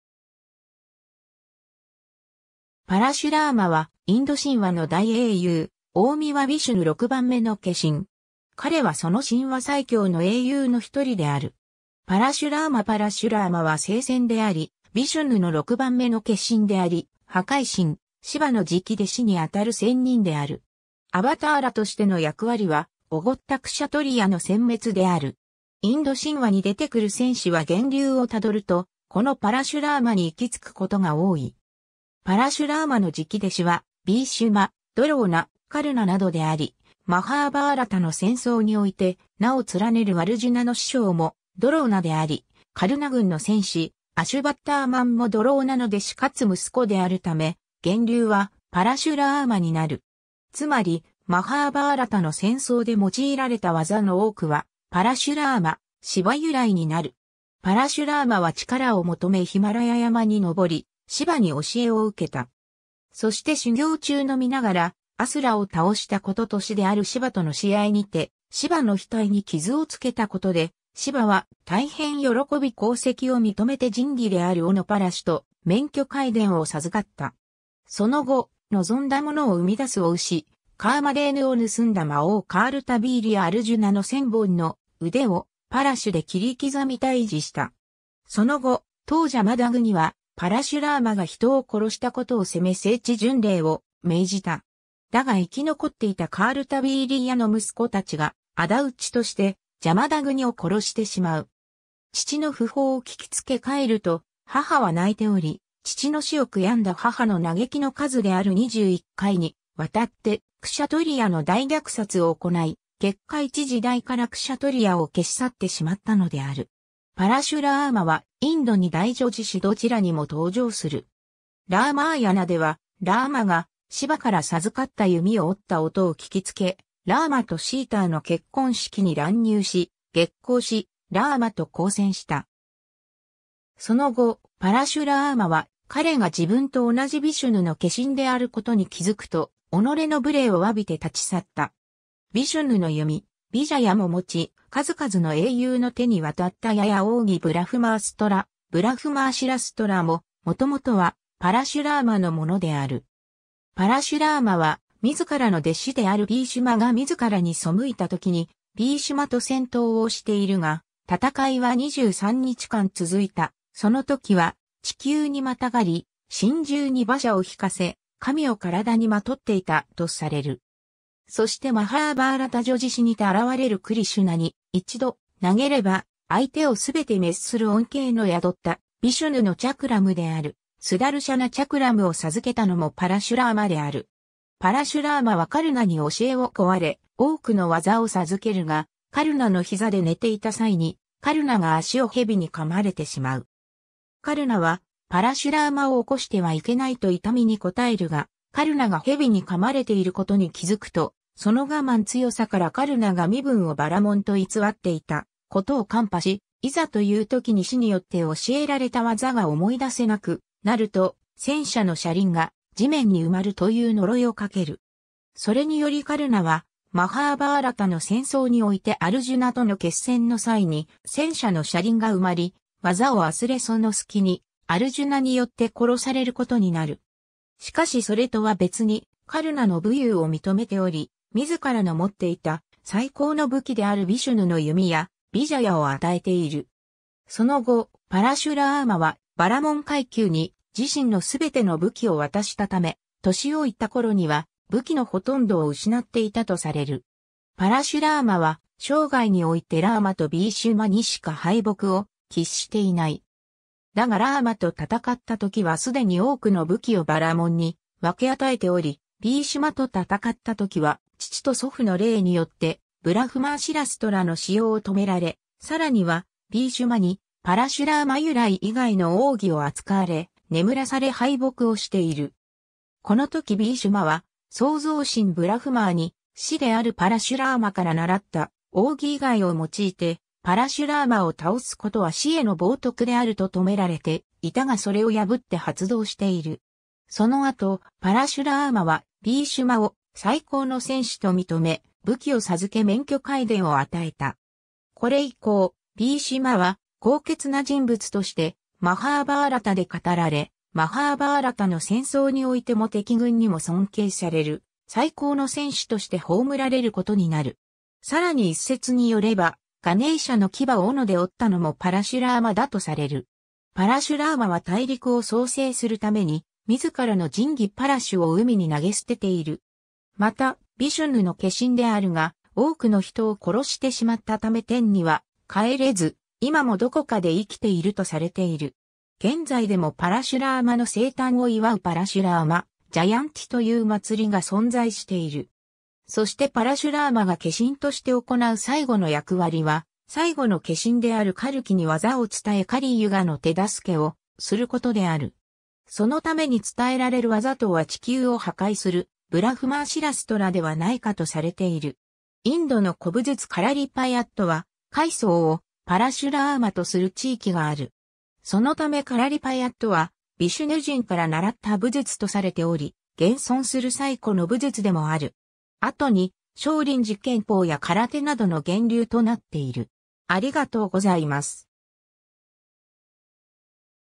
パラシュラーマ
パラシュラーマ（Parashurama サンスクリット語 ：परशुराम、 IAST ：Paraśurāma、斧を持つラーマ の意）は、インド神話の大英雄、大神ヴィシュヌ6番目の化身。
彼はその神話最強の英雄の一人である。
 パラシュラーマ（Raja Ravi Varma画） パラシュラーマは聖仙であり、ヴィシュヌの６番目の化身（アヴァターラ）であり、破壊神シヴァの直弟子にあたる仙人である。
アヴァターラとしての役割は「驕ったクシャトリヤの殲滅」である。
インド神話に出てくる戦士は源流を辿るとこのパラシュラーマに行き着くことが多い。
パラシュラーマの直弟子はビーシュマ、ドローナ、カルナなどであり、マハーバーラタの戦争において名を連ねるアルジュナの師匠もドローナであり、カルナ軍の戦士アシュヴァッターマンもドローナの弟子かつ息子であるため源流はパラシュラーマになる。
つまりマハーバーラタの戦争で用いられた技の多くはパラシュラーマ、シヴァ由来になる。
 パラシュラーマは力を求めヒマラヤ山に登りシヴァに教えを受けた。
そして修行中の身ながらアスラを倒したことと師であるシヴァとの試合にてシヴァの額に傷をつけたことで、シヴァは大変喜び功績を認めて神器である斧パラシュと免許皆伝を授かった。
その後、望んだものを産み出す牡牛カーマデーヌを盗んだ魔王カールタヴィーリヤ・アルジュナの千本の腕をパラシュで切り刻み退治した。
その後、父ジャマダグニはパラシュラーマが人を殺したことを責め聖地巡礼を命じた。
だが生き残っていたカールタヴィーリヤの息子たちが仇討ちとしてジャマダグニを殺してしまう。
父の訃報を聞きつけ帰ると母は泣いており、父の死を悔やんだ母の嘆きの数である21回に渡ってクシャトリヤの大虐殺を行い、結果一時代からクシャトリヤを消し去ってしまったのである。
 パラシュラーマはインド二大叙事詩どちらにも登場する。
ラーマーヤナでは、ラーマがシヴァから授かった弓を折った音を聞きつけラーマとシーターの結婚式に乱入し、激昂しラーマと交戦した。
その後、パラシュラーマは彼が自分と同じヴィシュヌの化身である事に気付くと、己の無礼を詫びて立ち去った。
 ヴィシュヌの弓ヴィジャヤも持ち、数々の英雄の手に渡った矢や奥義ブラフマーストラ、ブラフマーシラストラも、もともとはパラシュラーマのものである。
パラシュラーマは自らの弟子であるビーシュマが自らに背いたときにビーシュマと戦闘をしているが、戦いは23日間続いた、その時は「地球にまたがり」、「神獣に馬車を引かせ」、「神を体に纏っていた」とされる。
そしてマハーバーラタ叙事詩にて現れるクリシュナに「一度投げれば相手を全て滅する恩恵」の宿ったヴィシュヌのチャクラムであるスダルシャナ・チャクラムを授けたのもパラシュラーマである。
 パラシュラーマはカルナに教えを乞われ、多くの技を授けるが、カルナの膝で寝ていた際にカルナが足を蛇に咬まれてしまう。
カルナはパラシュラーマを起こしてはいけないと痛みに堪えるが、カルナが蛇に咬まれていることに気づくとその我慢強さからカルナが身分をバラモンと偽っていたことを看破し、「いざと言う時に師（パラシュラーマ）によって教えられた技が思い出せなくなる」と「戦車の車輪が地面に埋まる」という呪いをかける。
それによりカルナはマハーバーラタの戦争においてアルジュナとの決戦の際に戦車の車輪が埋まり、技を忘れその隙にアルジュナによって殺されることになる。
しかしそれとは別にカルナの武勇を認めており、自らの持っていた最高の武器であるヴィシュヌの弓矢ヴィジャヤを与えている。
 その後、パラシュラーマはバラモン階級に自身の全ての武器を渡したため、年老いた頃には武器のほとんどを失っていたとされる。
パラシュラーマは生涯においてラーマとビーシュマにしか敗北を喫していない。
だがラーマと戦ったときは既に多くの武器をバラモンに分け与えており、ビーシュマと戦ったときは父と祖父の霊によってブラフマーシラストラの使用を止められ、さらにはビーシュマにパラシュラーマ由来以外の奥義を扱われ眠らされ敗北をしている。
このときビーシュマは創造神ブラフマーに「師であるパラシュラーマから習った奥義以外を用いてパラシュラーマを倒すことは師への冒涜である」と止められていたがそれを破って発動している。
その後パラシュラーマはビーシュマを「最高の戦士」と認め、武器を授け免許皆伝を与えた。
これ以降ビーシュマ...